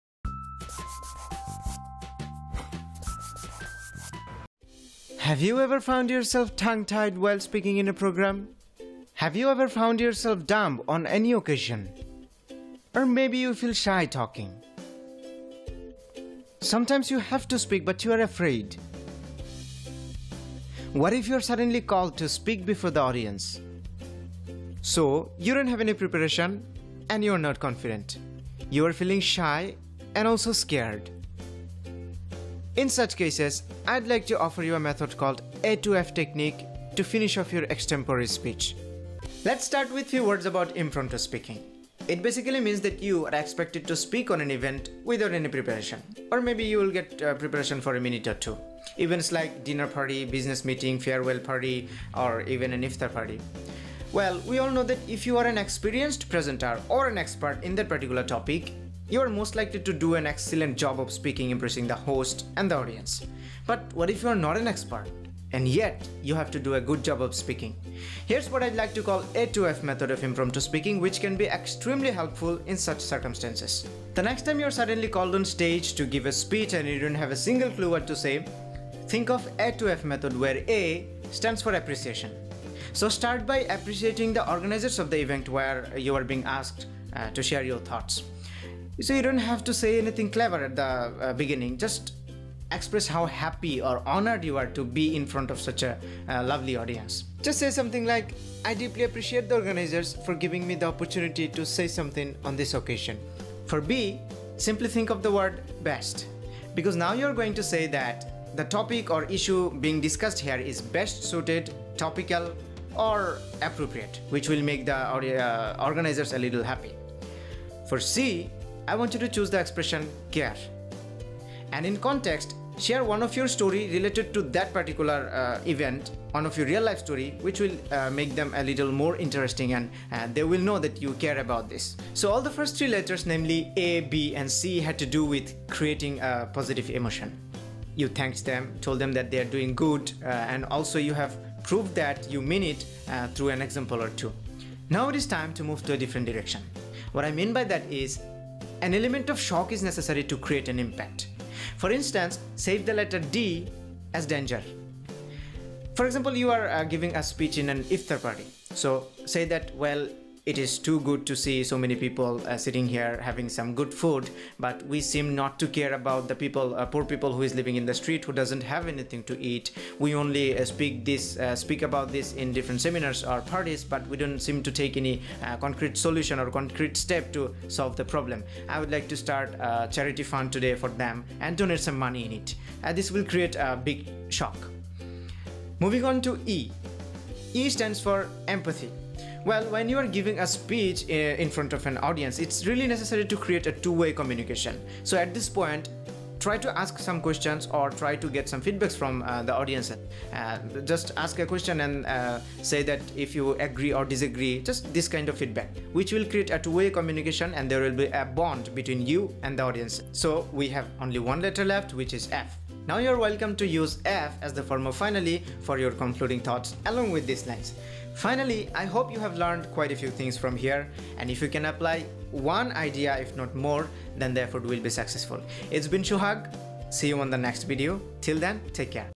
A to F Method of Sudden/Impromptu/Extempore Speech
Impromptu speaking means giving speech without any preparation. If you are not an expert presenter then you might find it difficult to speak before an audience without preparation.
A to F method is an easy solution for you.

A-Appreciation
B-Best Suited
C-Care
D-Danger
E-Empathy
F-Finally

So keep this method in your head, you may find it helpful during an extempore speech.

Shibli Shahriar I Qualified and Certified University Faculty and Master Trainer I